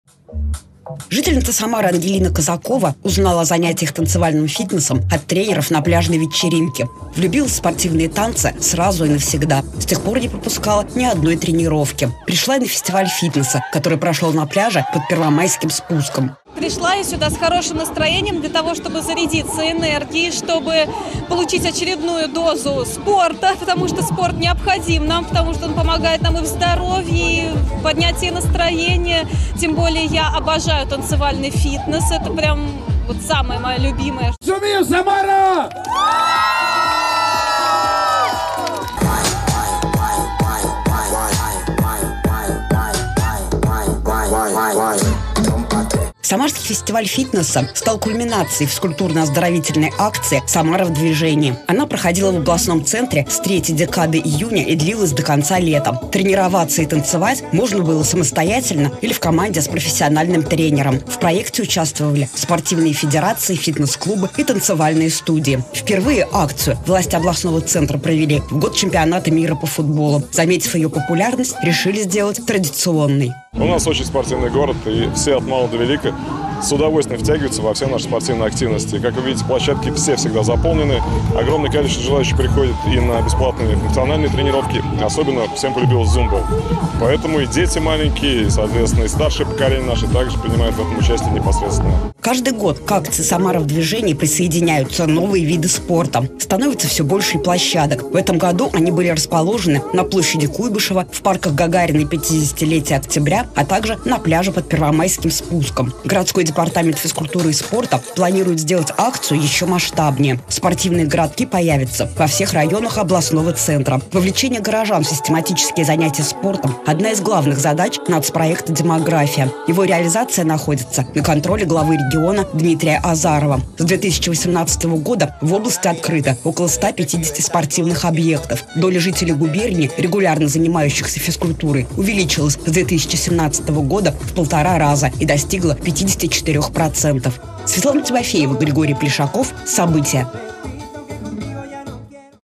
В Самаре на набережной прошел фестиваль фитнеса
Тысячи самарцев и гостей областного центра приняли участие в акции "Самара в движении". Кульминацией акции стал фестиваль фитнеса, который прошел в эти выходные на пляже под Первомайским спуском. Как "зажигали" гости спортивного праздника, расскажем в нашем следующем репортаже.